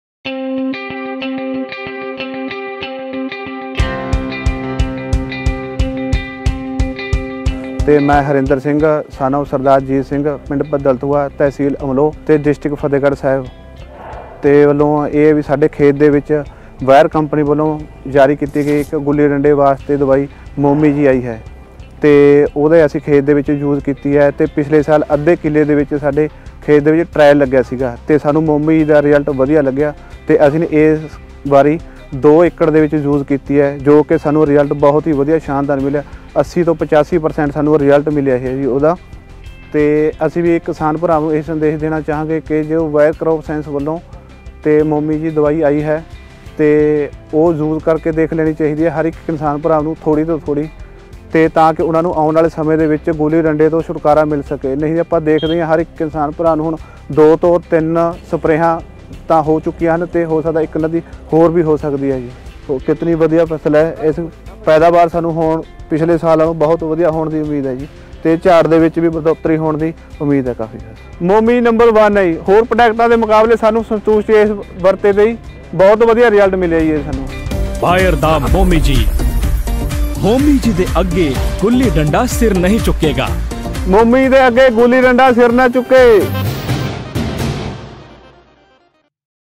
Best Weedicide for Phalaris Minor: Momiji Bayer | Bayer Crop Science India
Meet Harinder Singh, a wheat farmer from Padaltua, Amloh, Fatehgarh Sahib, Punjab sprayed Momiji and got 80 to 85% control over phalaris minor weed keeping the crop safe. He had tried it for only half an acre of his field last year, but now he has planned to use it on the complete field. He saved expenses on additional sprays, which he used to do in the past.